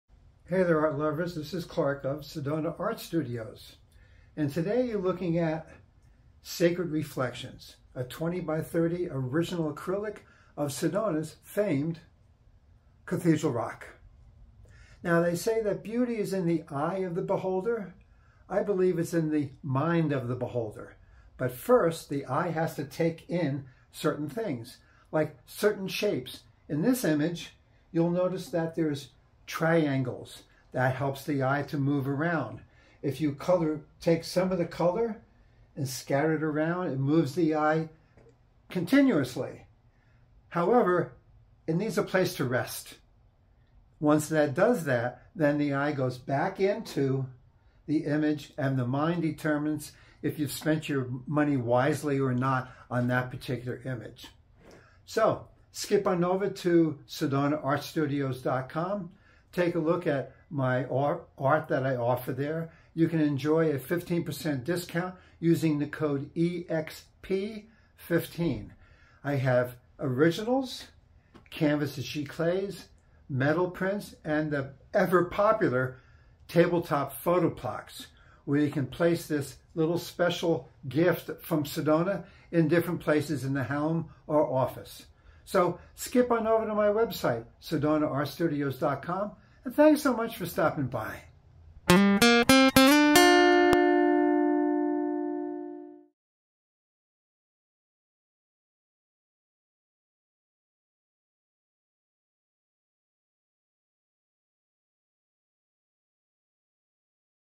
Sacred Reflections Artwork
Sacred Reflections invites you to reflect on yourself at Cathedral Rock, a mystical and stunning Vortex in Sedona. Nature's wonders remind us to slow down, take a breath, and reflect on how we respond to life.
Discover This Sedona Vortex. Nature and art always remind us to stop and appreciate the beauty around us. Take in the stunning Sedona vortex landscape painting, representing life's essence. To experience Sedona and its vortices, book a healing journey tour with Vishali.